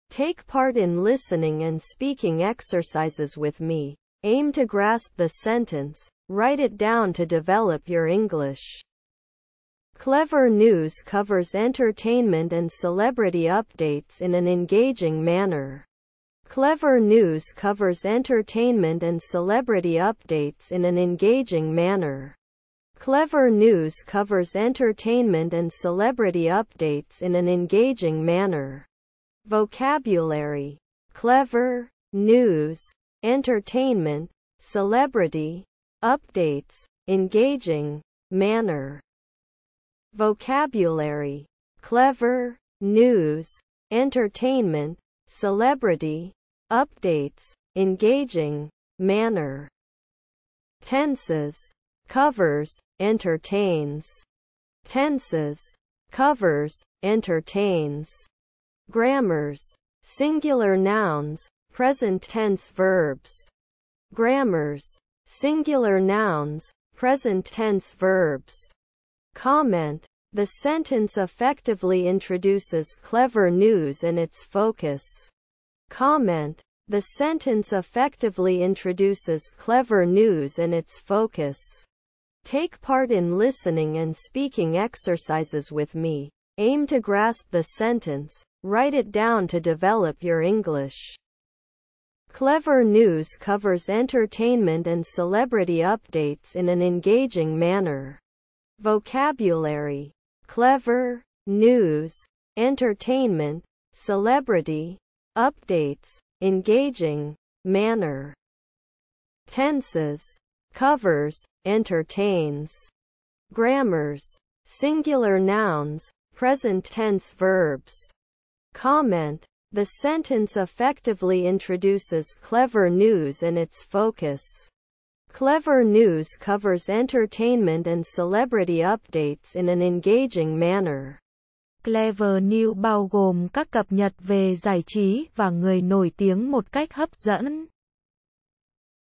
Clevver News covers entertainment and celebrity updates in an engaging manner.
Take part in listening and speaking exercises with me, aim to grasp the sentence, write it down to develop your English.

Vocabulary: Clevver, News, entertainment, celebrity, updates, engaging, manner.

Tenses: covers, entertains.

Grammars: Singular nouns, present tense verbs.

Comment: The sentence effectively introduces Clevver News and its focus.